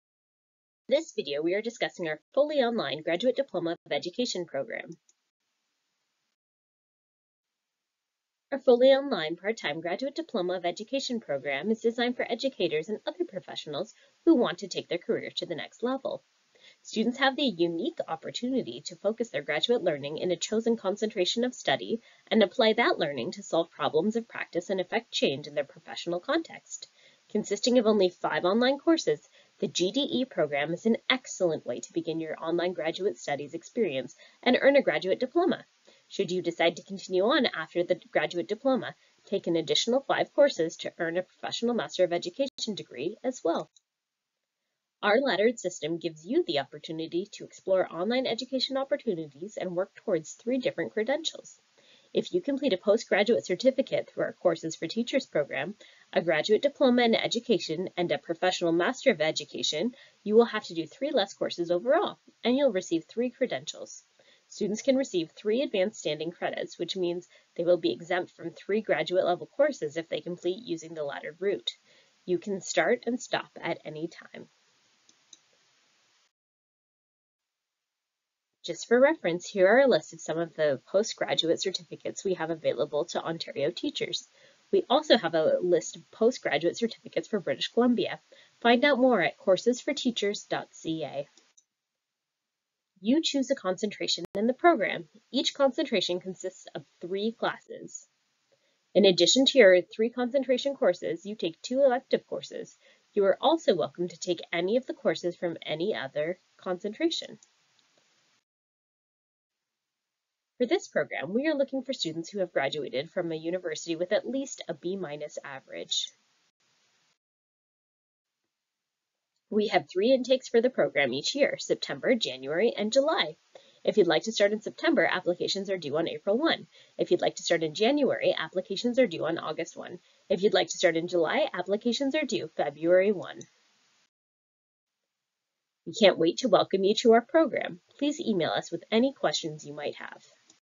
Queen's Faculty of Education's Graduate Diploma of Education
The GDE program offers a flexible and personalized approach to post-graduate studies in education. All GDE courses run asynchronously online, which means professionals can participate from anywhere in the world without disrupting their busy schedules.